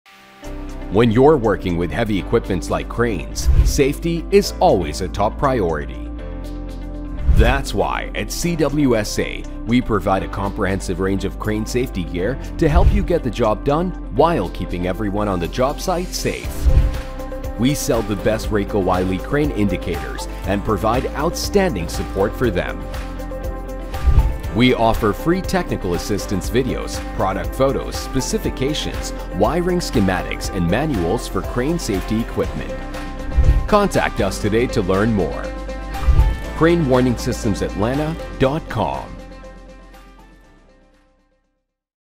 Keep Your Job Site Safe with CWSA's Crane Safety Gear and RaycoWylie Crane Indicators
At CWSA, we understand that safety is a top priority when working with heavy equipment like cranes. That's why we offer a comprehensive range of crane safety gear and sell the best RaycoWylie crane indicators to help you get the job done while keeping everyone on the job site safe. We provide free technical assistance videos, product photos, specifications, wiring schematics, and manuals to ensure that you have all the information you need to make informed decisions about your crane safety gear.